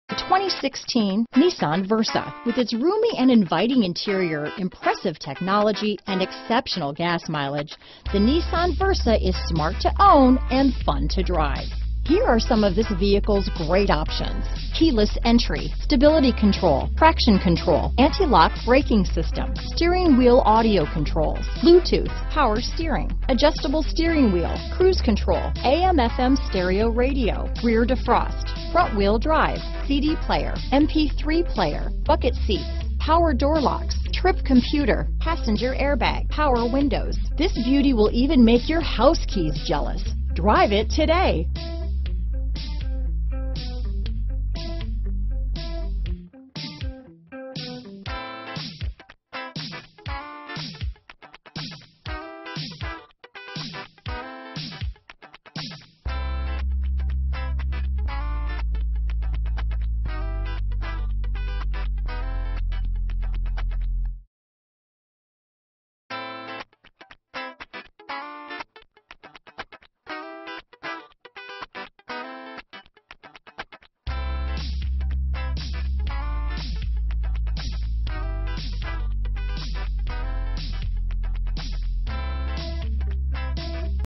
2016 Nissan Versa San Antonio, Austin, Houston, New Braunfels, Helotes, TX N62615
2016 Nissan Versa

Gunn Nissan
12838 San Predro

Gets Great Gas Mileage: 40 MPG Hwy*** Safety equipment includes: ABS, Traction control, Curtain airbags, Passenger Airbag, Stability control...Other features include: Bluetooth, Power locks, Power windows, CVT Transmission, Air conditioning... The 2016 is the Nissan Versa Sedan is fun, efficient, roomy and comes with a bit of style. Subtle chrome grille accents and chrome door handles, sleek lines, and broad shoulders make the Versa Sedan stand out from other cars in its class. With a spacious interior the Versa offers a surprising amount of leg and headroom to seat five people comfortable. With 14.9 cubic feet of room, Versa's trunk lets you bring along everything you need, and keep it safe and with 60/40-split fold-down rear seats you have flexibility to carry people and your cargo. With a 1.6L engine you can choose from a choice of 3 different transmissions including a 5-speed manual, a 4-speed automatic, or the efficient Xtronic CVT (Continuously Variable Transmission). With the Xtronic CVT you can achieve up to 40MPG hwy and it also wears the PUREDRIVE badge, demonstrating Nissan's commitment to creating an eco-friendly lineup of lower-emission vehicles. The available NissanConnect with Navigation and Mobile Apps is equipped with innovative features like hands-free phone capability, streaming audio, a USB connection port, enhanced voice recognition, and navigation system with a 5.8-inch color display that serves up points of interest like restaurants, gas stations, and ATMs. Back up with confidence with the Versa's optional RearView Monitor.